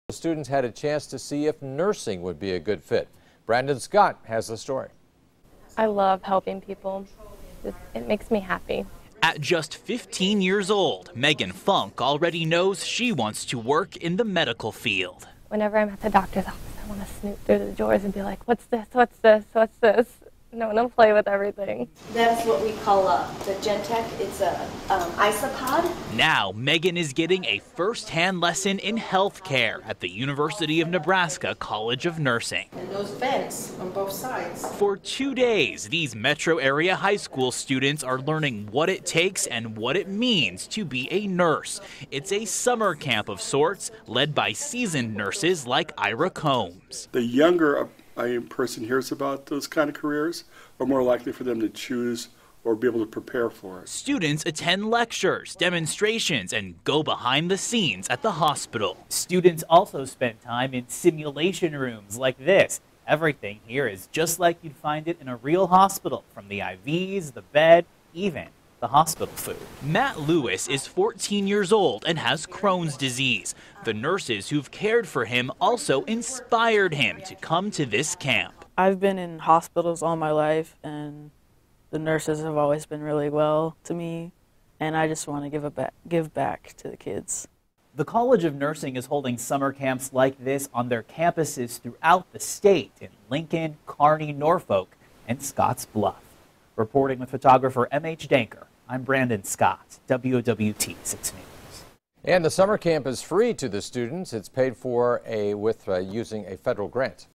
Nurse Camp
A GROUP OF HIGH SCHOOL STUDENTS HAD THE CHANCE TO SEE IF NURSING WOULD BE A GOOD FIT. BRANDON SCOTT HAS THE STORY.